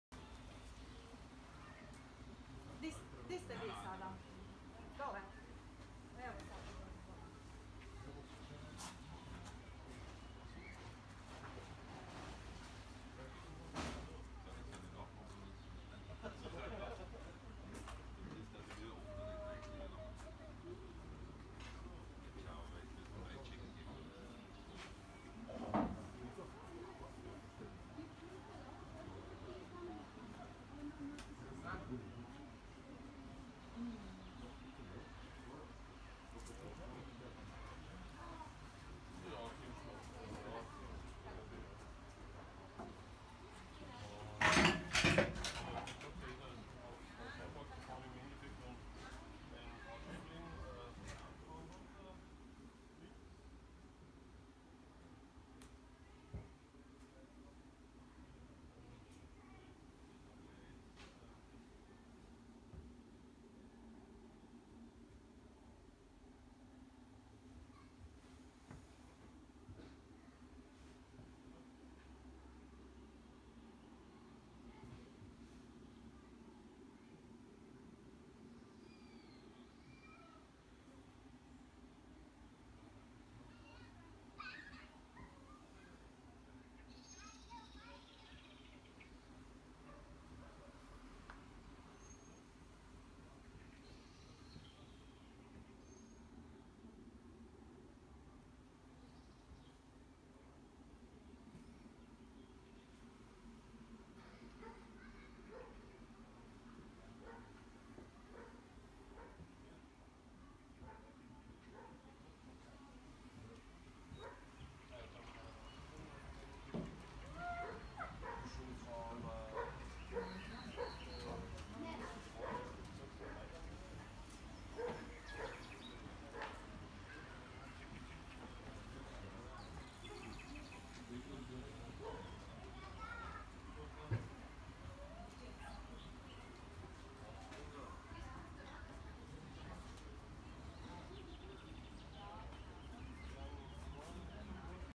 Camp Polidor, Croatia
Camp Polidor with Adria mobile homes where you can spend your next holidays.